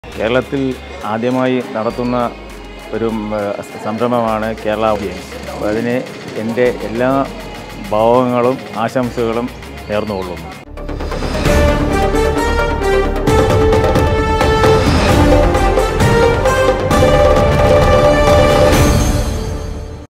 കേരള ഗെയിംസിന് ആശംസകളുമായി ഹോക്കി കോച്ച് എസ്.ജയകുമാർ | S. JAYAKUMAR WISHES | KERALA GAMES 2022
For the first time in the State, Kerala Olympic Association is conducting the KERALA OLYMPIC GAMES 2022 (KOG - 2022) from 29th April - 10th May 2022 at various prominent venues in the capital city of Kerala, Trivandrum. 24 different disciplines have been scheduled as part of the first KOG 2022.Sports persons from all 14 district will be competing in KOG 2022